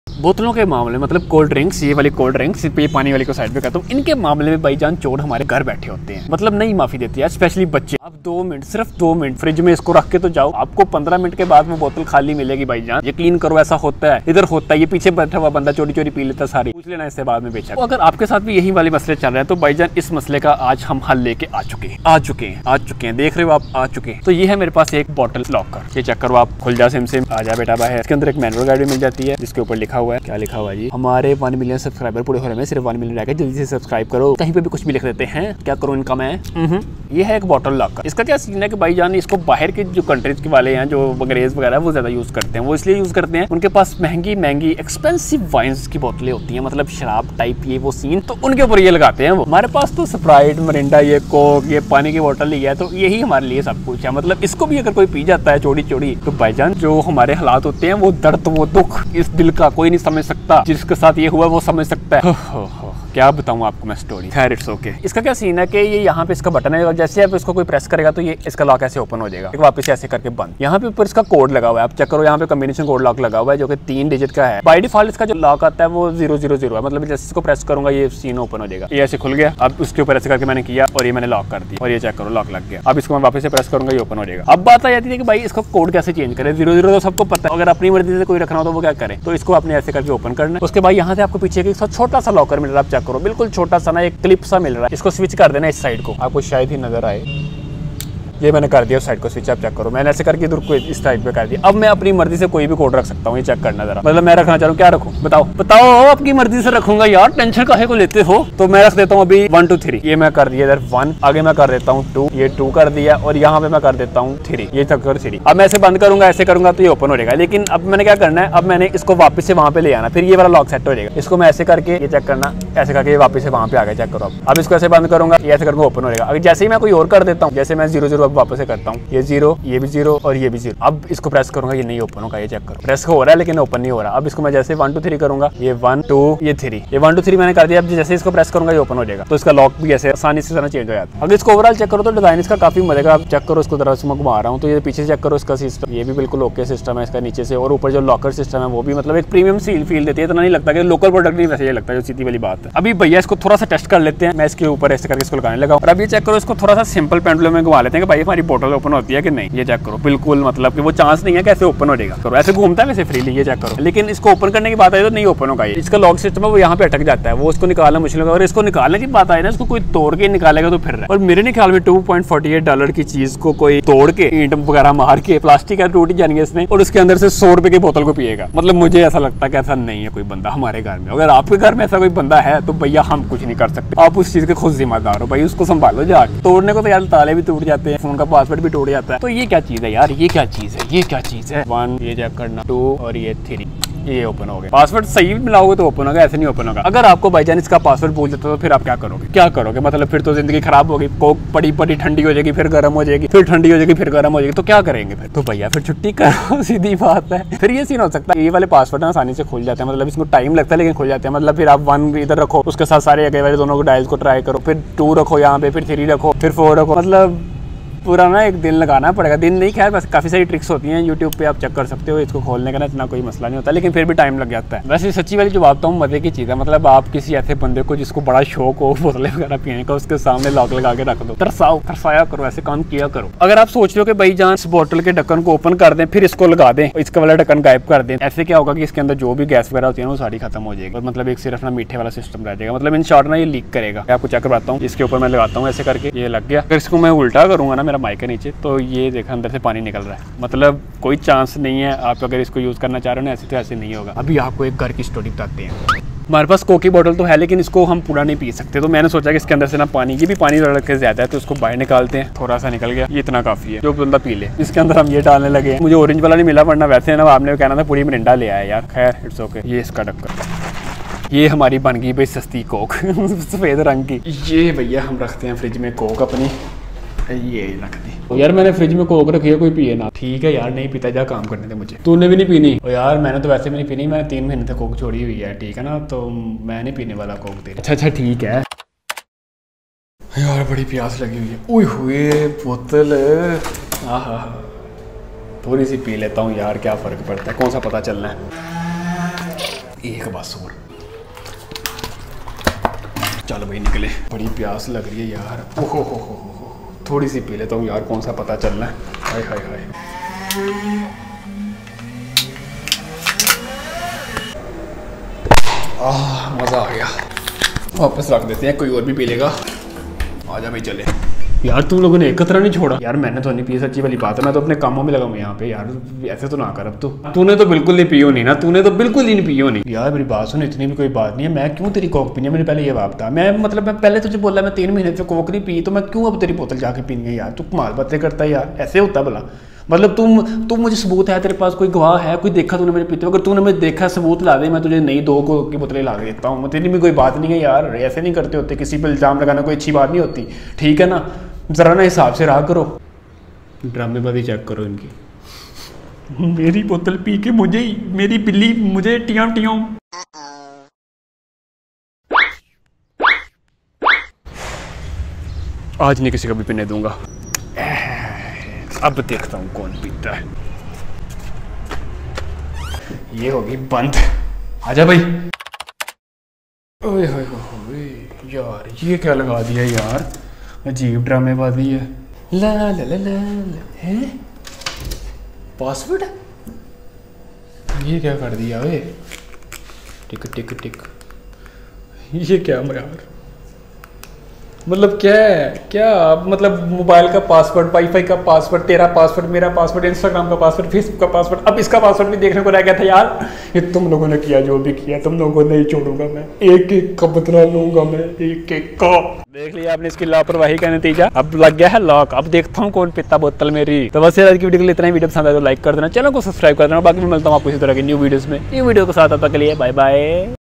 3 Digits Bottle Locker | How To Change Password
KAK Plastic Bottle Password Lock Combination Lock Wine Stopper Vacuum Plug Device Preservation For Furniture Lock Hardware

Specifications:

Material: Plastic
Size: 43mm*43mm
Function: To help keep the wine fresh
and keep the wine can't be stolen
Environmental Standards: Europe&Nickel,
Lead,Cadmium free

Password Setting Method:

1st step: Press and hold the silver round button on top.
2nd Step: Switch the clip to opposite side which is just below or inside the locker.
3rd step:Adjust the number on top ( initial password:000) to a new password you want to set.
4th step: Release the button again to opposing side when you set the password, password setting will be finished.